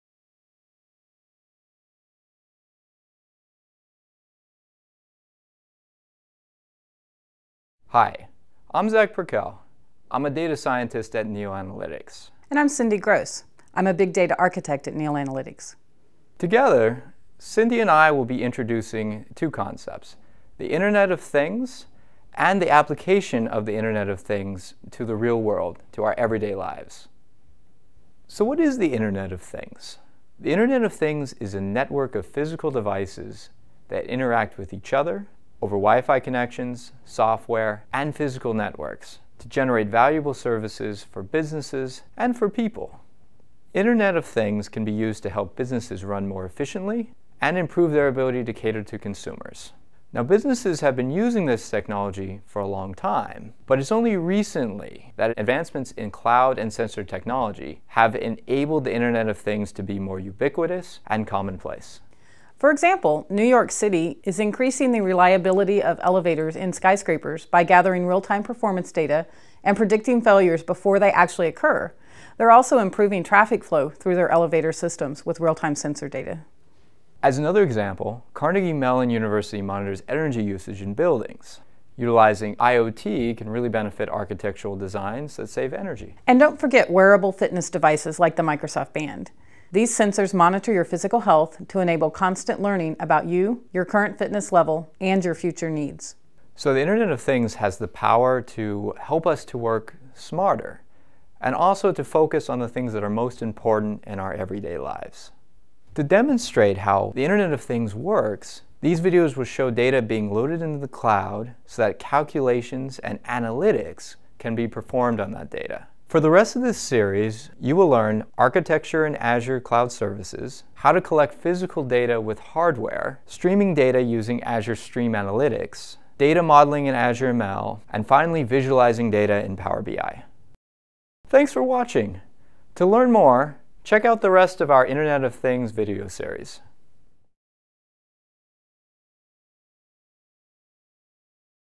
01 Internet of Things Video Series Introduction
Hello and welcome to the introduction video to our Internet of Things Video Series. In this series of videos we will show you the essentials in Azure Machine Learning, Data Collection, and Stream Analytics to help you gain important information for your growing business. Join us, and we will show you how to use the Internet of Things to help your business make great steps forward. Be sure to check out the rest of videos of our Internet of Things Series.